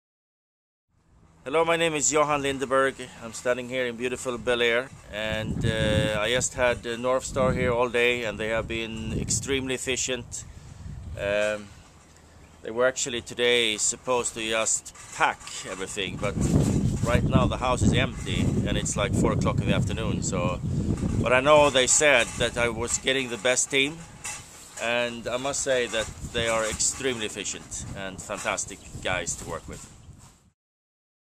Video Testimonial | Johan Lindeberg Moved with NorthStar Moving Company
Johan Lindeberg moved Interstate with NorthStar Moving from Bel Air, CA to New York, NY and used our Packing and Storage Services (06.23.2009). Check out what he had to say about his experience:  "Hello my name is Johan and I am standing here in beautiful Bel-Air and I just had NorthStar Moving here all day and they have been extremely efficient. They were actually were supposed to just pack everything, but right now everything is empty and it’s 4 o’clock in the afternoon but I know they said that I am working with the best team and I must say they are extremely efficient and fantastic guys to work with."